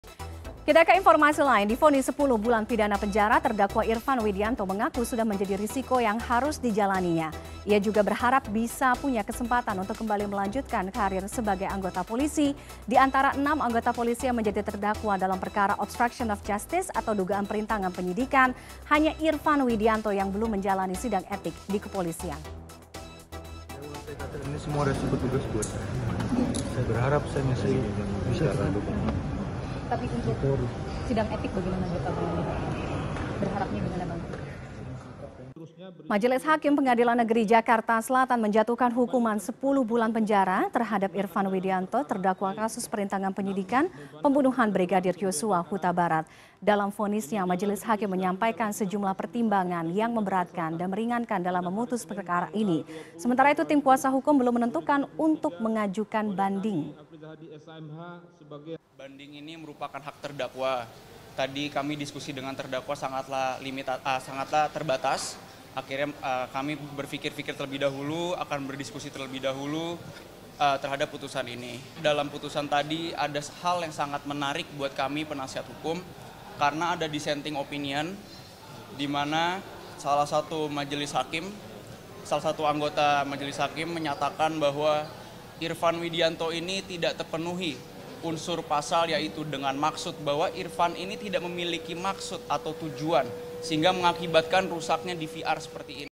Divonis 10 Bulan Penjara, Irfan Berharap Tetap Jadi Polisi
Di vonis 10 bulan pidana penjara, terdakwa Irfan Widyanto mengaku, sudah menjadi risiko yang harus dijalaninya. Ia juga berharap bisa punya kesempatan, untuk kembali melanjutkan karir sebagai anggota polisi.